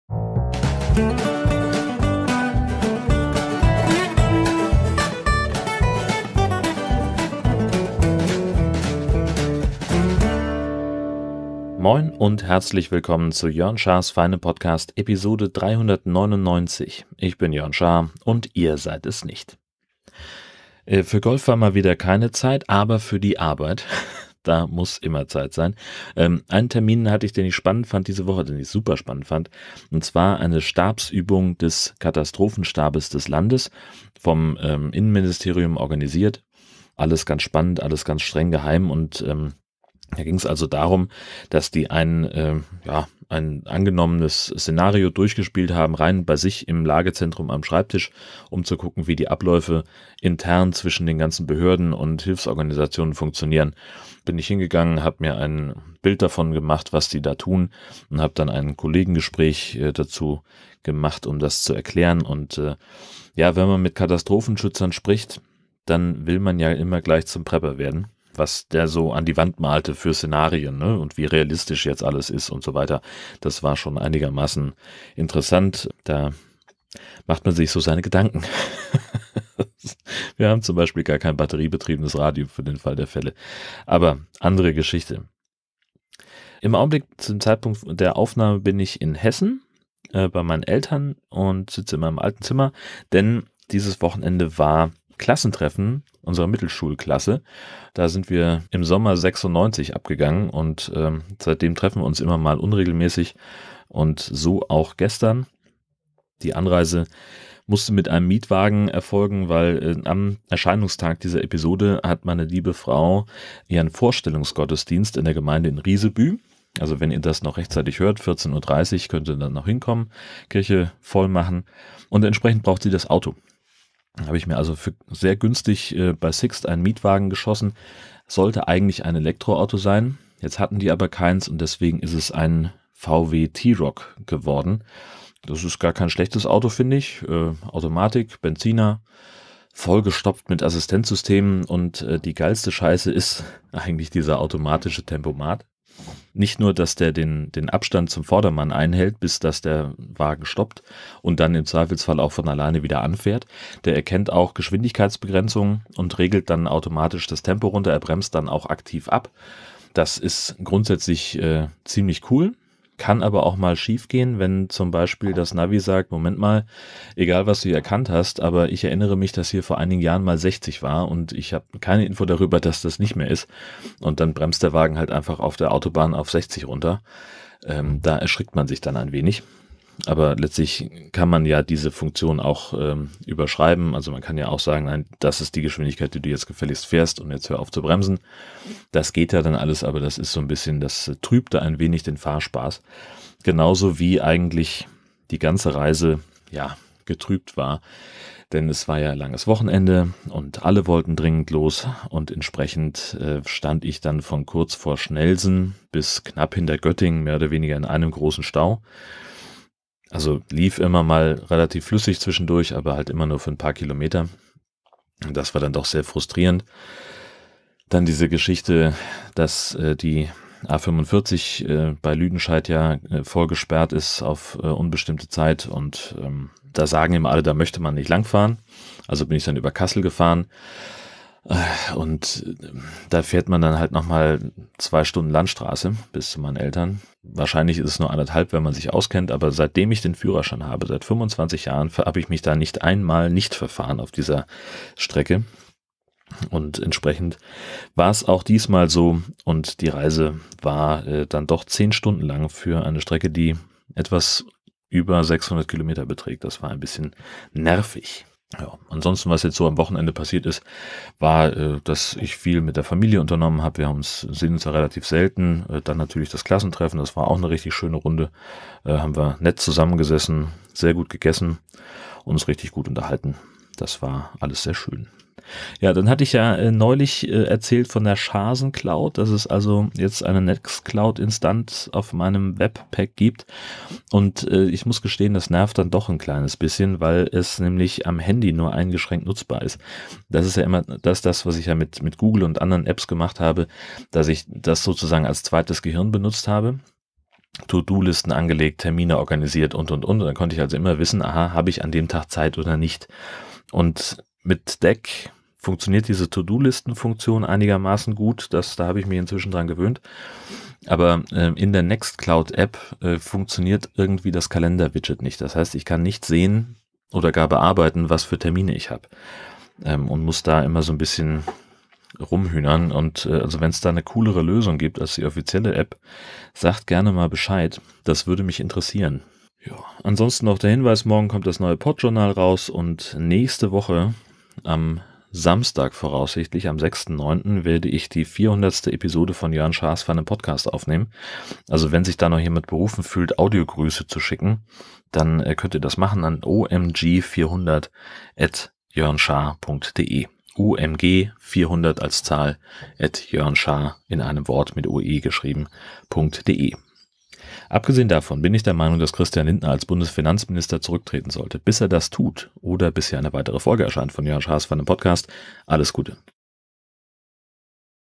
JSFP399: Leihwagenodyssee
Dienstlich war ich diese Woche unter anderem bei einer Übung des Landeskatastrophenschutzstabes und das war ganz spannend. Privat bin ich gerade in Hessen bei meinen Eltern und das ist eigentlich ganz schön, nur die Anreise war - sagen wir mal - problematisch. Und die Schaarsencloud nervt mich dann doch dezent.

Chapters:
00:00:00 JSFP399: Leihwagenodyssee
00:00:11 Intro
00:00:20 Arbeitswoche
00:01:26 Besuch in Hessen
00:04:35 Niederschlag aus der Schaarsencloud
00:05:29 Podjournal und JSFP400 ante portas
00:05:57 Abschied